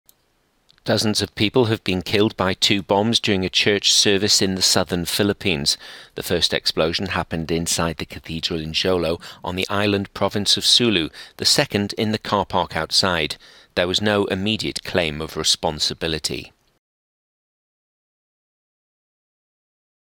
Double bomb explosion in Philippines kills at least 27
At least 27 people have been killed by two bombs during during a church service in the southern Philippines. …